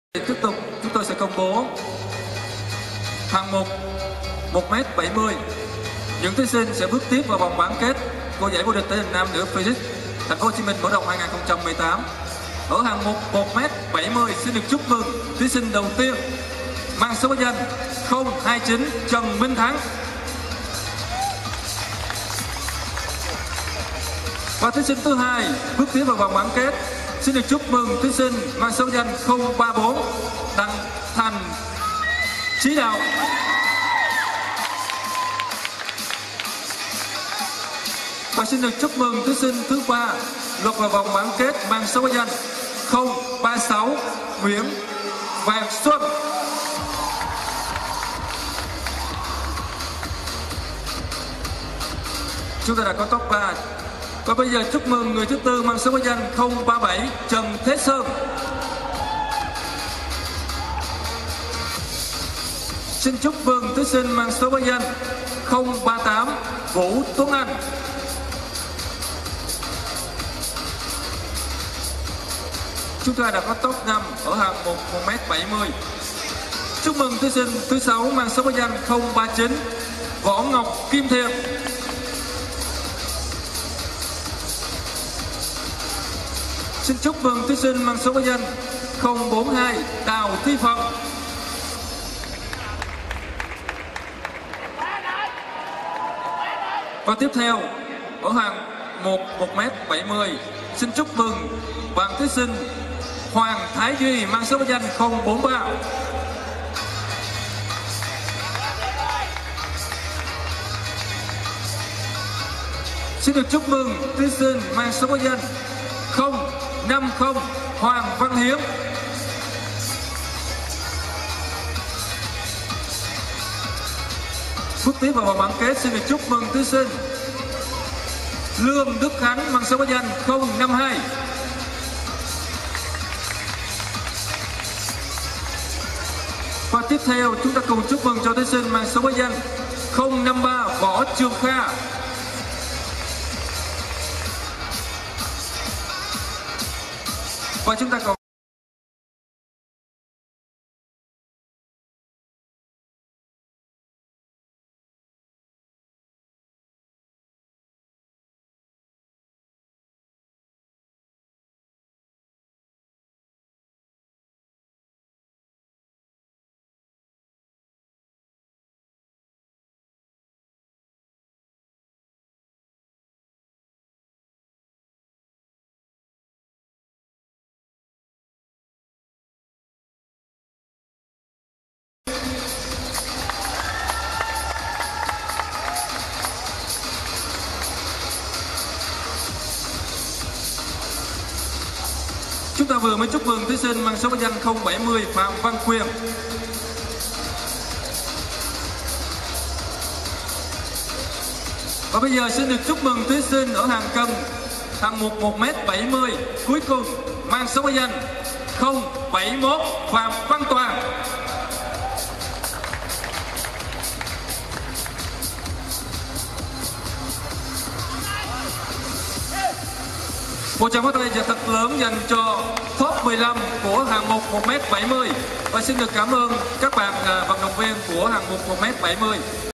Vận động viên Vietgym quận 7 lọt top 15 fitness models xuất sắc nhất | Viet Gym
Tại 🏆GIẢI THỂ HÌNH NAM - NỮ PHYSIQUE TP.HCM MỞ RỘNG 2018 với sự tham gia của 192 vận động viên đến từ cả nước, Vận động viên Võ Trường Kha - HLV hệ thống VietGym lọt top 15 fitness models xuất sắc nhất.

Xin chúc mừng các anh đã đại diện cho thanh thiếu niên quận 7 thắp lên ngọn lửa đam mê. Nơi nào có ý chí ắt nơi đó sẽ có con đường.

- Lê Văn Trưởng
- Võ Trường Kha
- Trần Văn Quyết
- Nguyễn Công Đạt